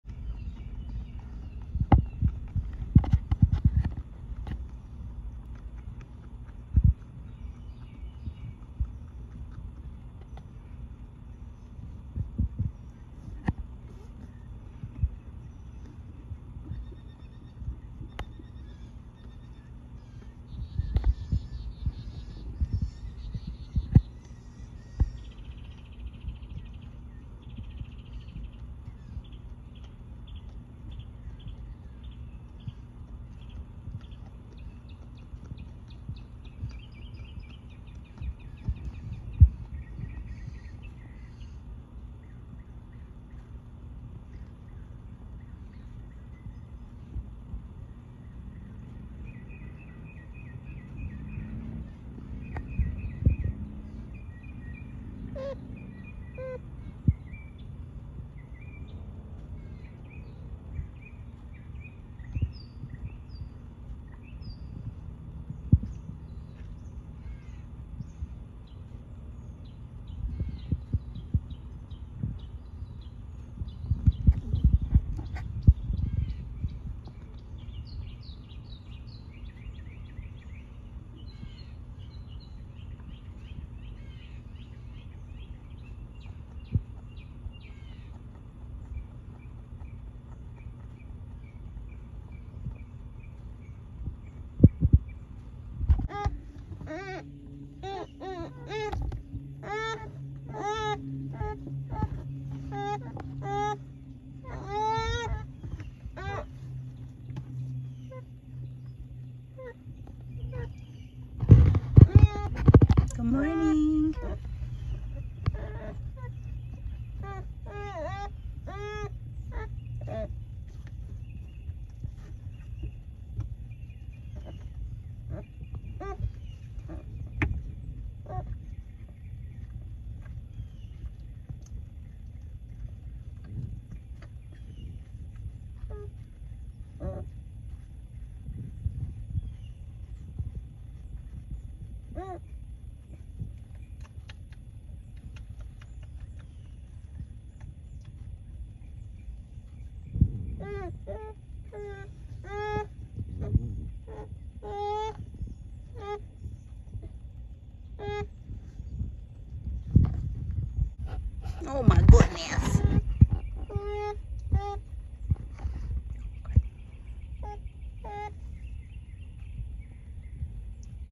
Baby beavers grooming
Tater and Turnip have moved to the big enclosure so Tulip can spend time outside. In true teenage fashion, Tater and Turnip slept in and were grumpy about waking up. Tulip had some great swim practice this morning that I’ll post in a separate video.

Anyone wanting to support our efforts can do so at the following links. Thank you!!